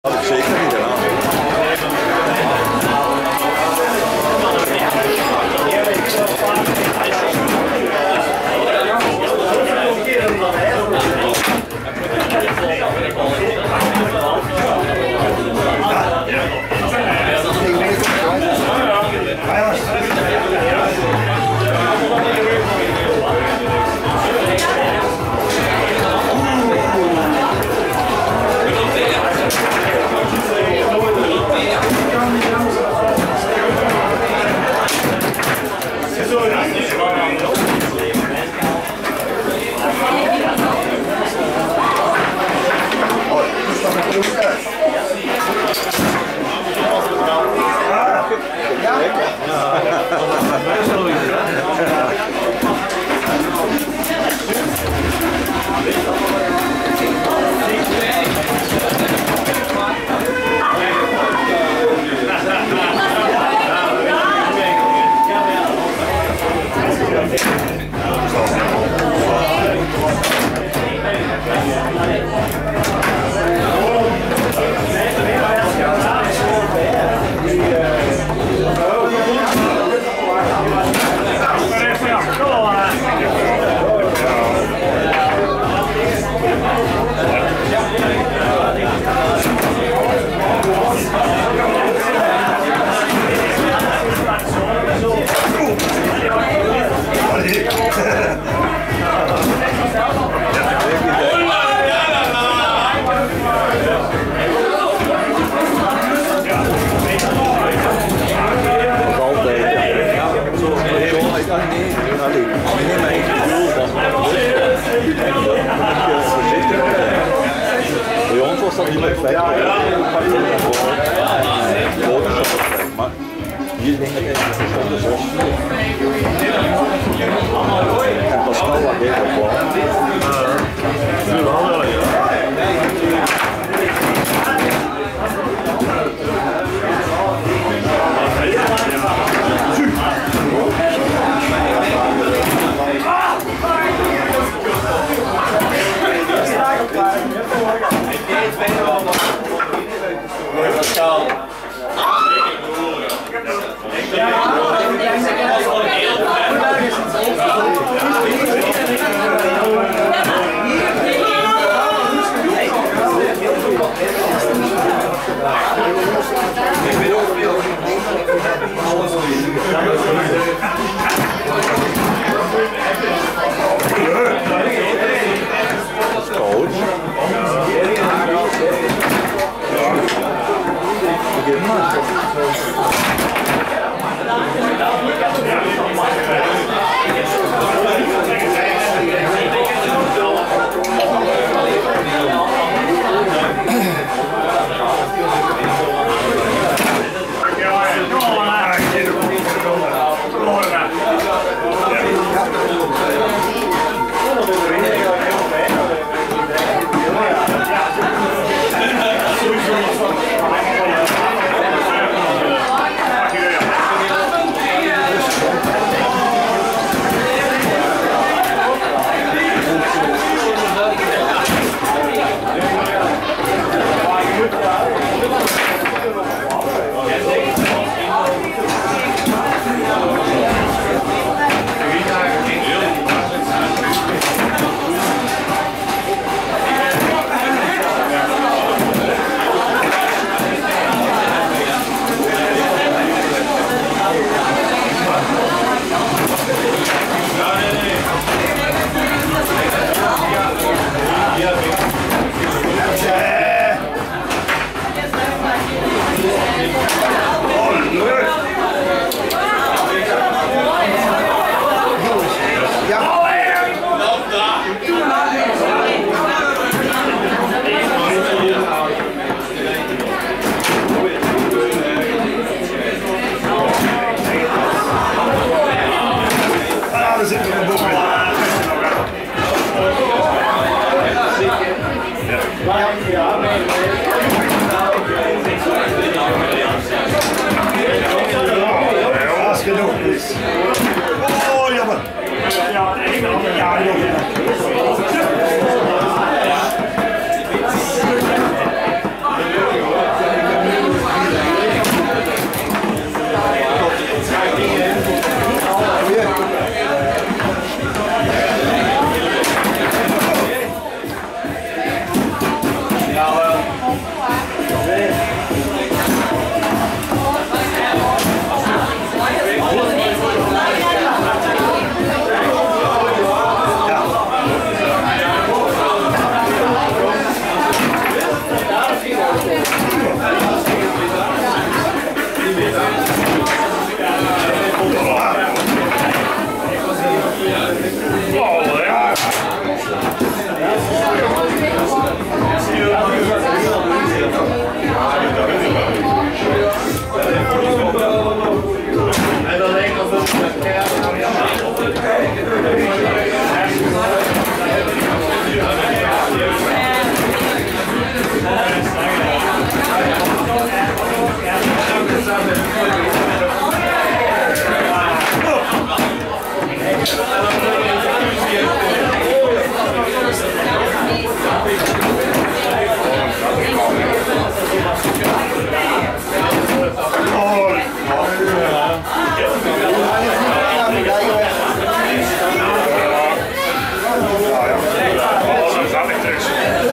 Liquidoshooters - Cosmos - beker 170120 4
Pascal Duchateau en Vincent Uyttebroeck in 5de set tegen Maurice Hauben en Kevin Stevens - Guido Carolus

Foosball Competition in Belgium, CBK- Regio Oost

kicker,  minifoot, Tischfußball, tafelvoetbal, futbolín metegol biliardino taca Table soccer

Belgische competitie 2de klasse